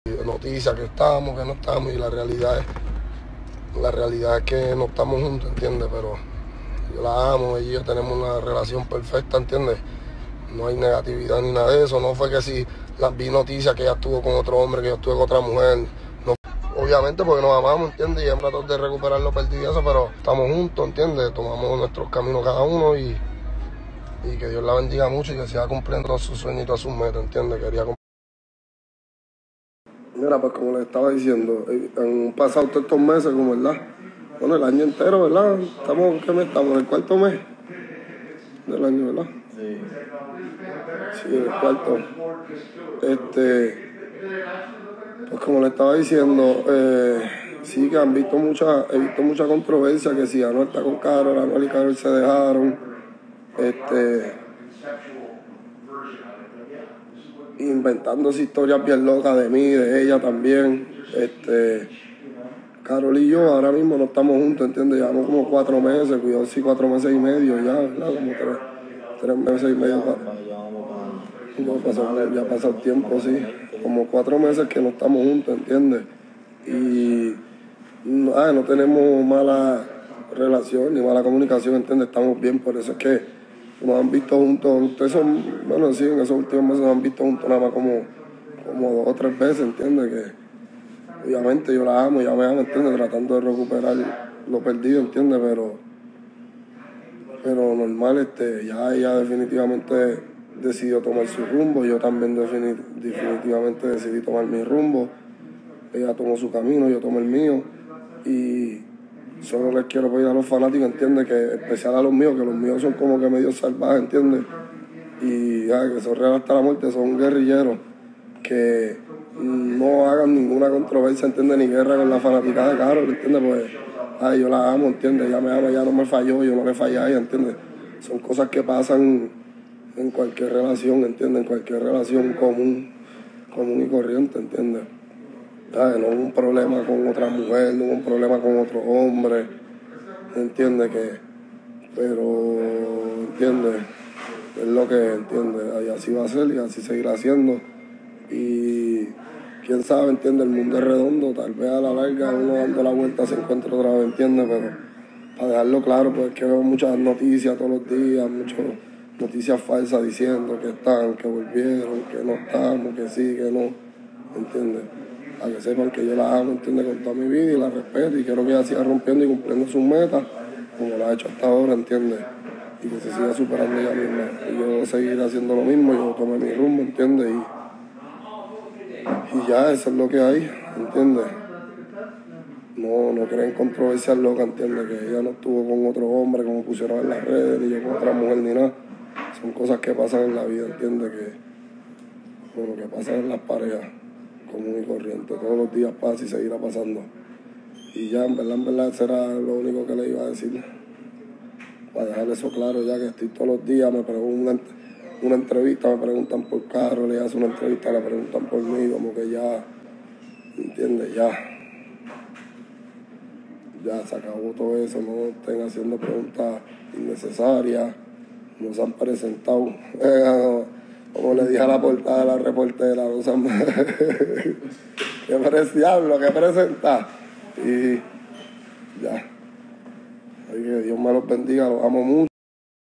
ANUEL CONFIRMA SEPARACION CON KAROL G 💔😭 "YO AUN LA AMO" (INSTAGRAM LIVE COMPLETO)
Digital distribution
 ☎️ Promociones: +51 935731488